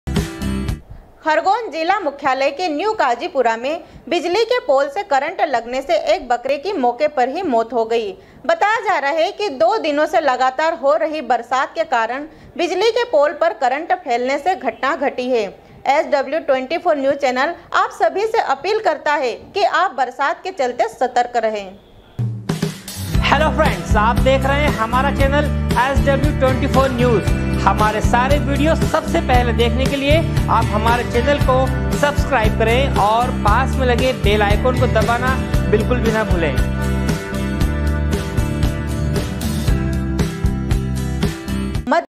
sw24news khargone, 09.08.2019
खरगोन जिले मुख्यालय के न्यू काजीपुरा में बिजली के पोल से करंट लगने से एक बकरे की मौके पर ही मौत हो गयी। एस डब्ल्यु 24 न्यूज चैनल आप सभी से अपील करता है कि आप बरसात के चलते सतर्क रहेंsw24news